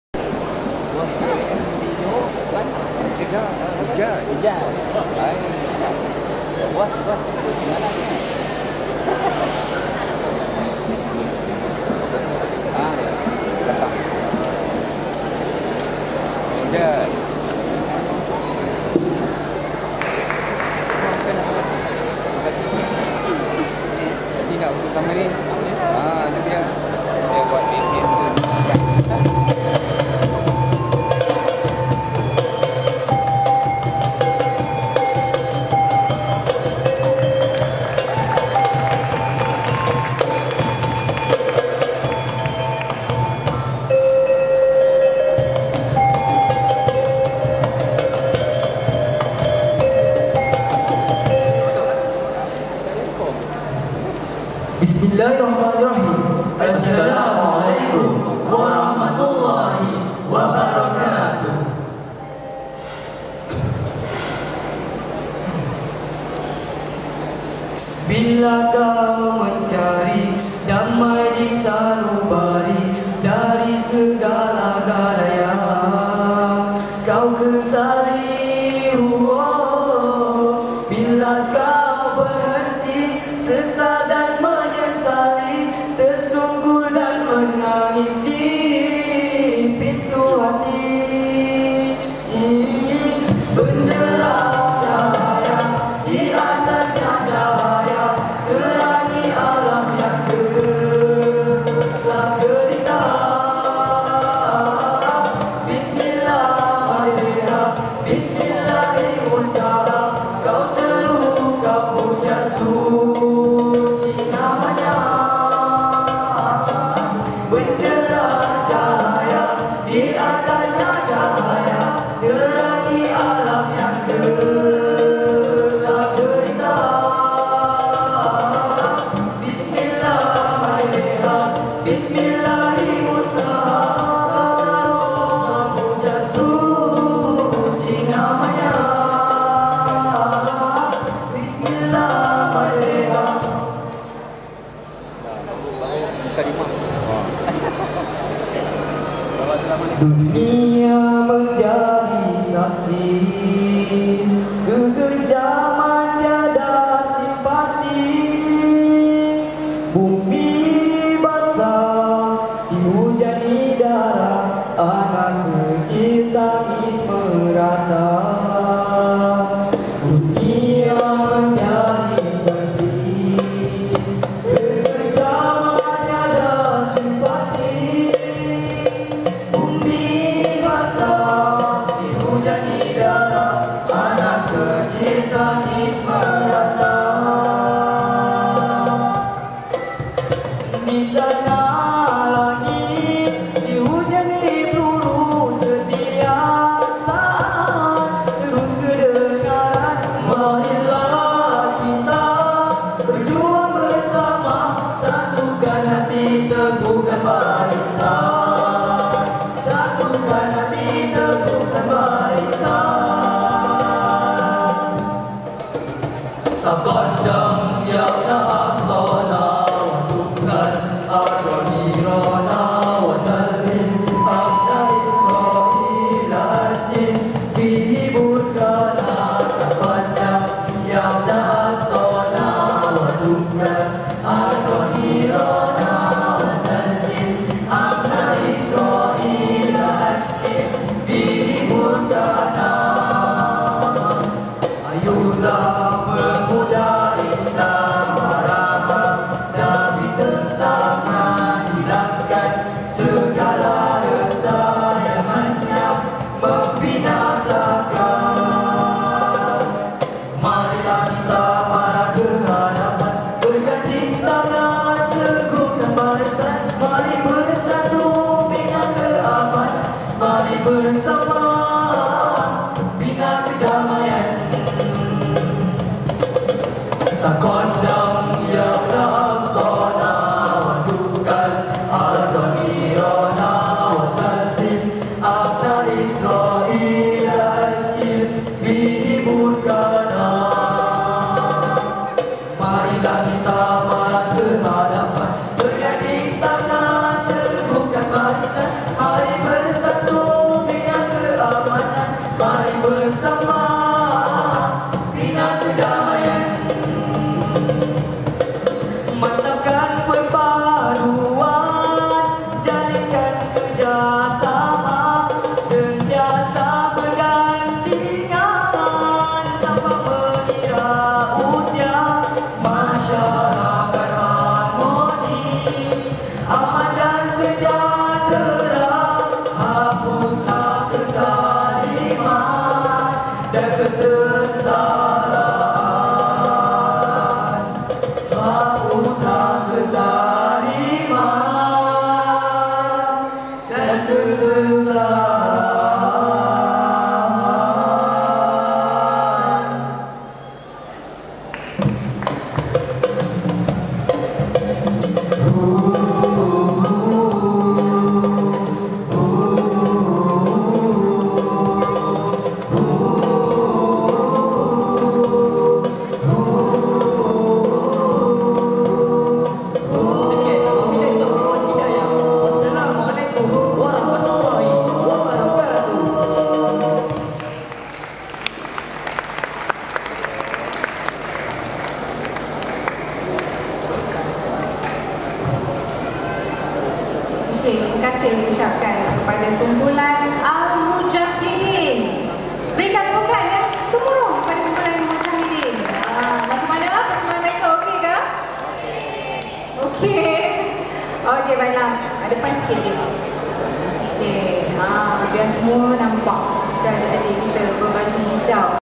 Bintang Nasyid Mu'min Johor 2015 - Al-Mujahidin
Giliran keempat, kumpulan Al-Mujahidin dari SMK Ayer Tawar, Kota Tinggi.

Bintang Nasyid Mu'min Musim ke-7
Peringkat Negeri Johor
4 April 2015

Johan - Izdan (SMKA Maahad Muar)
Naib Johan - Soutunnajah (SMKA Johor Bahru)
Ketiga - Raudhah (SMK Dato Penggawa Timur, Masai)

Solo Lelaki Terbaik - Izdan (SMKA Maahad Muar)
Solo Perempuan Terbaik - Raudhah (SMK Dato Penggawa Timur, Masai)
Susunan Perkusi Terbaik - Soutunnajah (SMKA Johor Bahru)
Persembahan Terbaik - Izdan (SMKA Maahad Muar)
Kostum Terbaik - Izdan (SMKA Maahad Muar)

Tahniah buat semua pemenang dan peserta